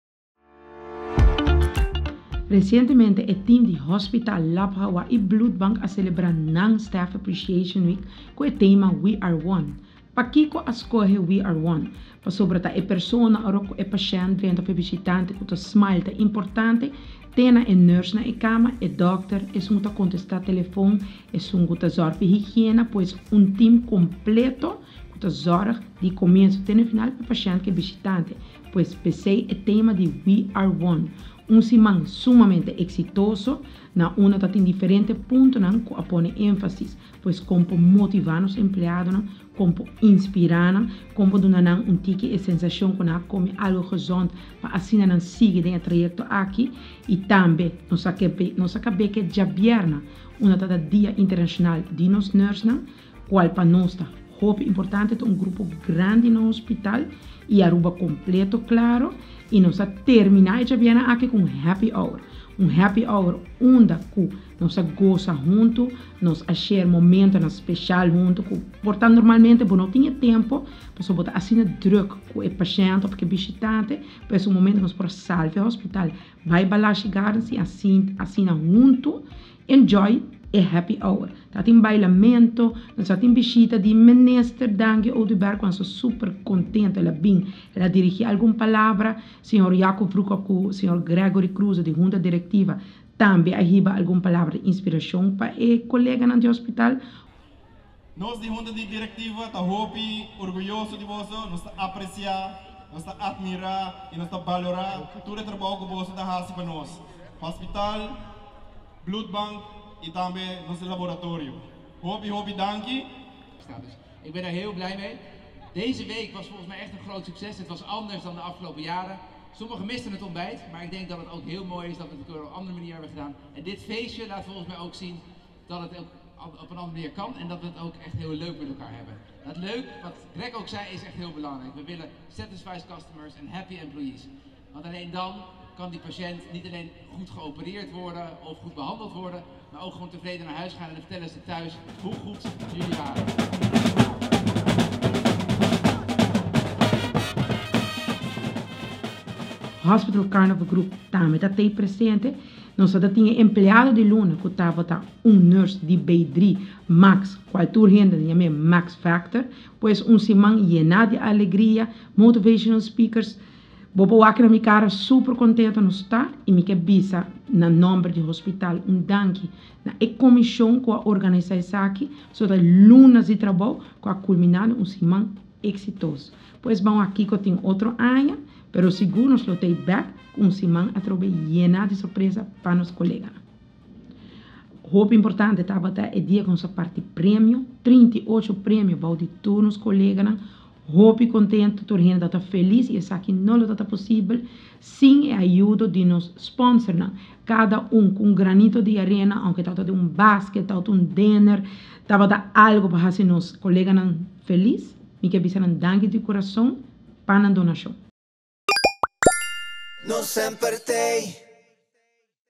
Den cuadro di Siman di Enfermeria Hospital a tene su Staff Appreciation Week
Recientemente a tuma luga HOH, LABHOH y Bloedbank su Staff Appreciation Week 2023 cual a culmina riba dia internacional di enfermeria.